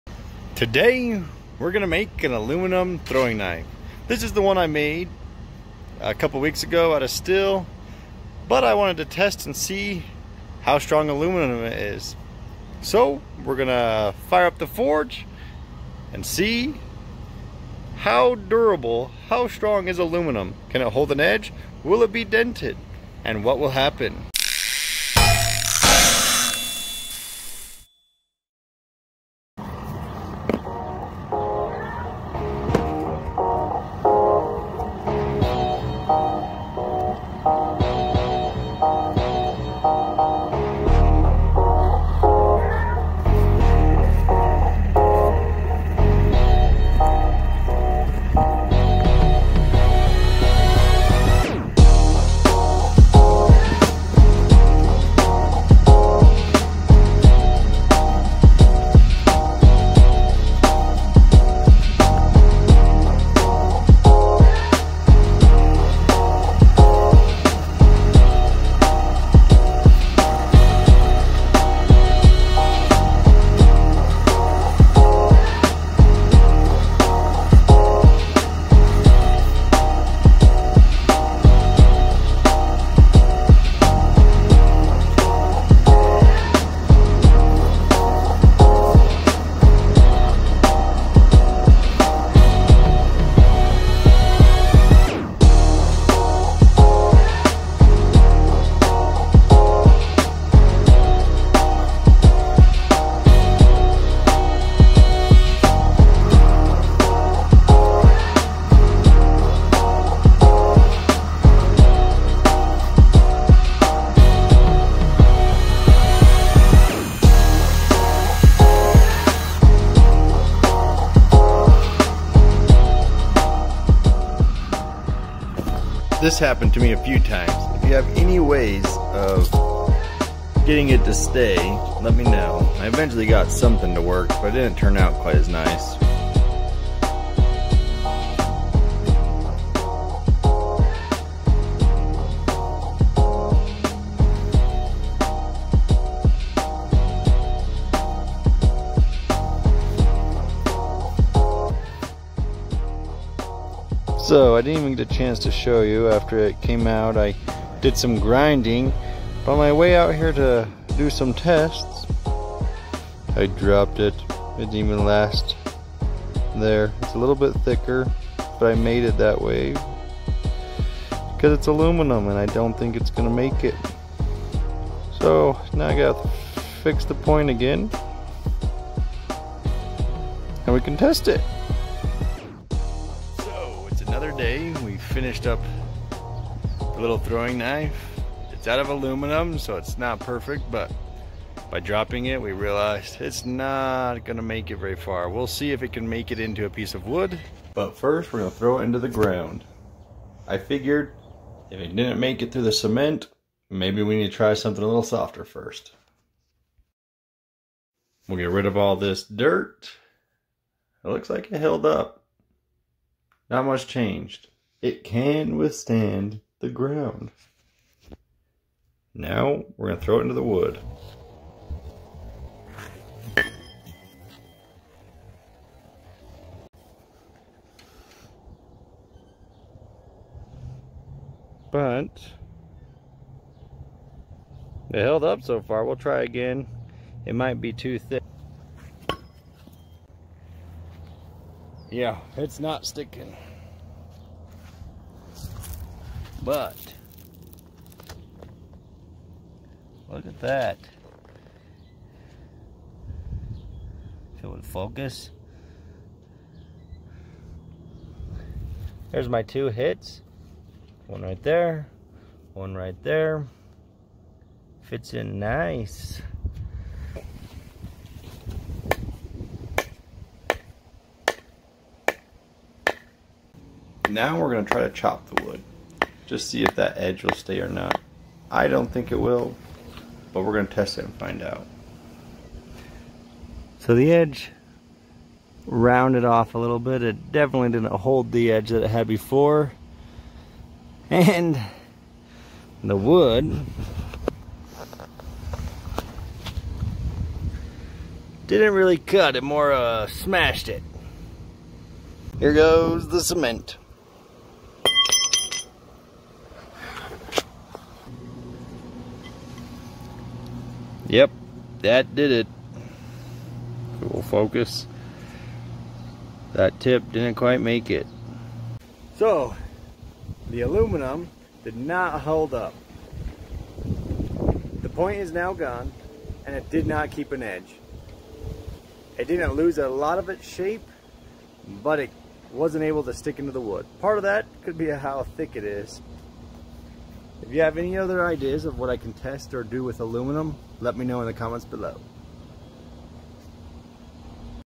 How Strong is Aluminum - Casting a Throwing Knife
Today we're going to make an aluminum throwing knife and test how strong it is. Aluminum is a softer metal so how much can it with stand. Can it hold an edge? I cover all of that and the process of making the throwing knife.